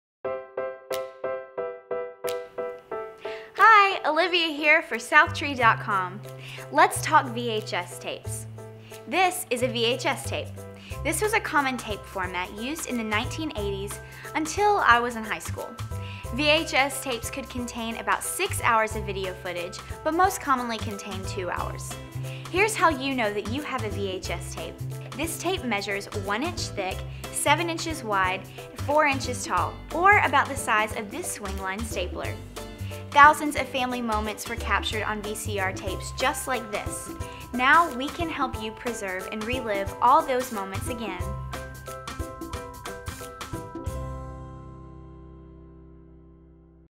Southtree U: VHS Tapes
Our amazingly cool team at Southtree.com delve into the confusing world of analog videotapes, film, and photos to educate you on these aging formats. This clips looks at converting VHS to DVD and shows how you know if you have VHS tapes to preserve.